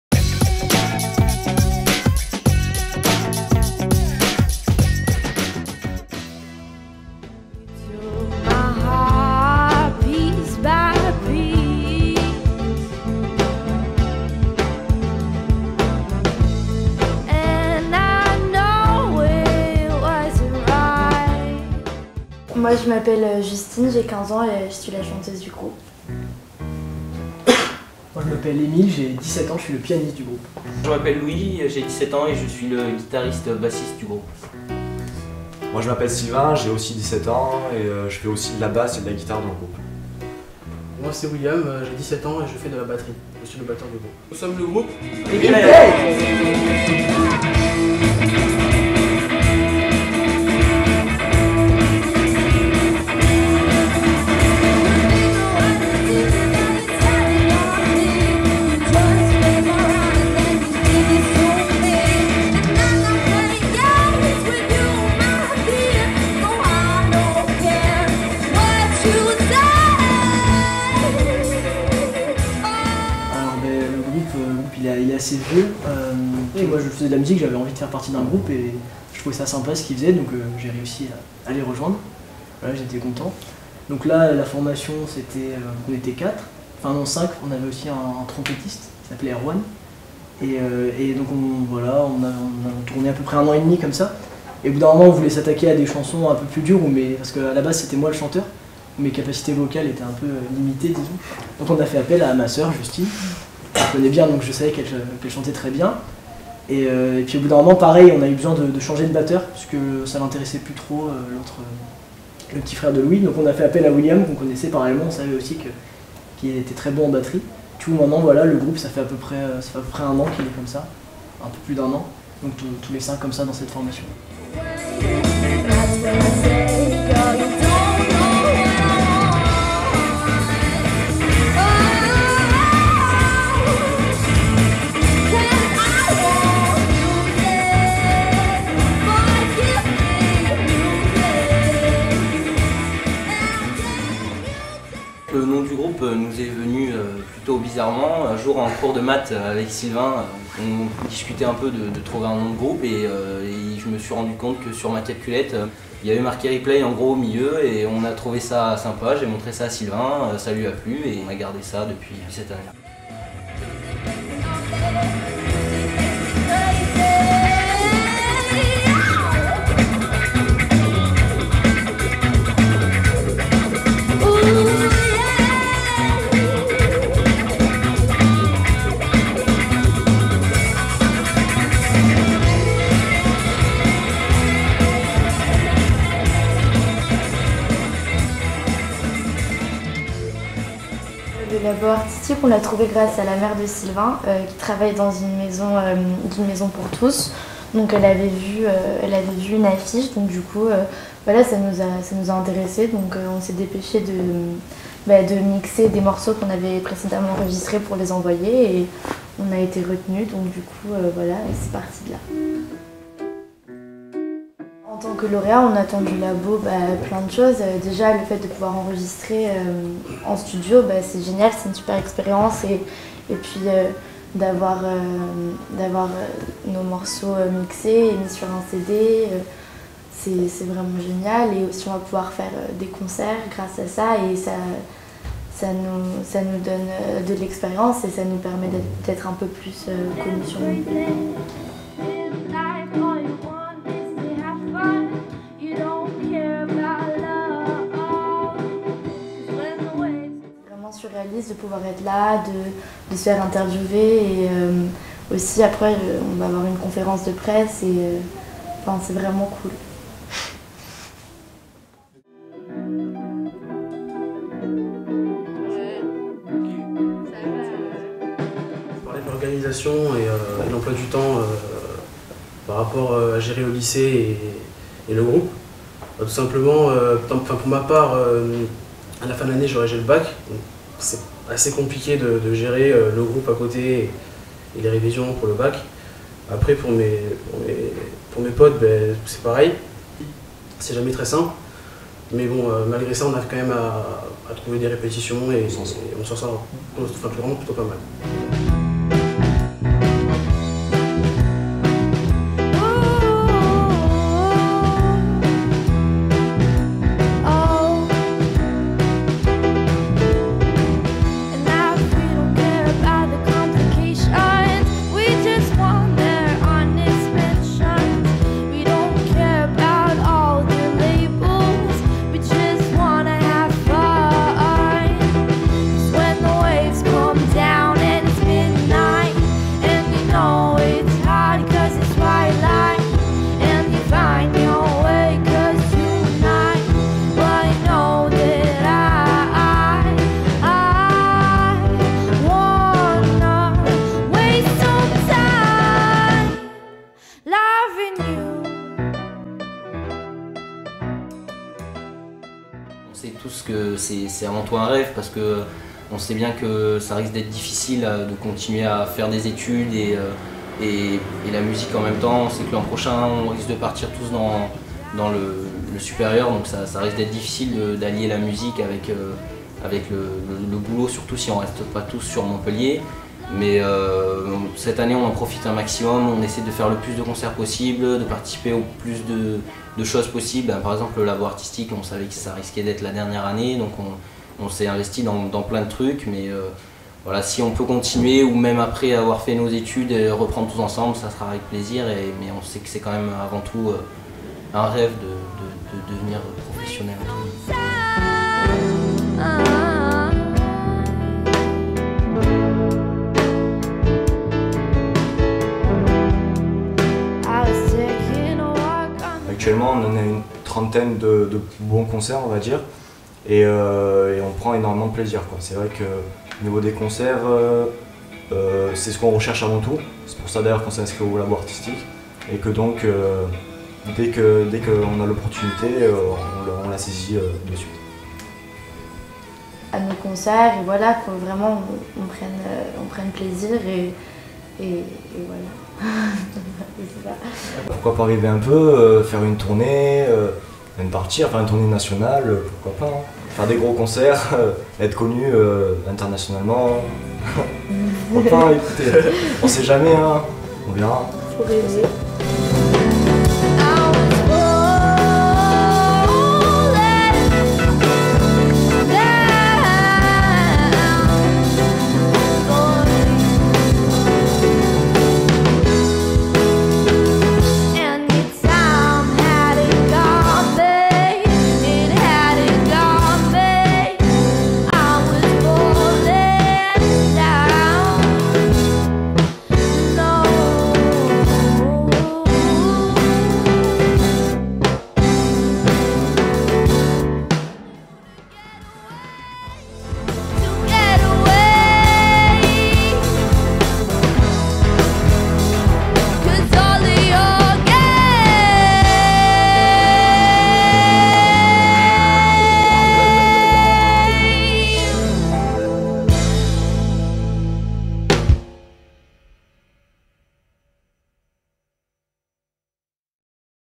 Interview du Groupe Montpellierain Replay lauréat 2014 du Laboratoire Artistique
Interview du goupe rock Replay, lauréat du Laboratoire Artistique édition 2014.
Nous les avons reçus dans notre studio.